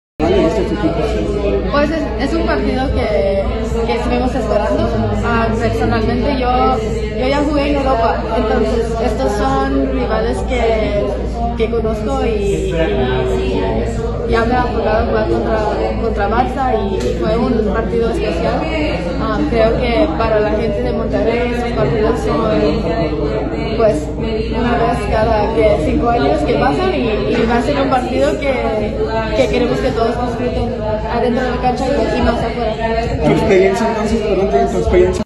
Anika Rodríguez afirma que ya conoce al Barcelona y RealMadrid
“Yo ya jugué en Europa, son rivales que conozco, para la gente de Monterrey será especial”, aseguró Anika Rodríguez, lateral de Tigres Femenil, previo al encuentro del Real Madrid.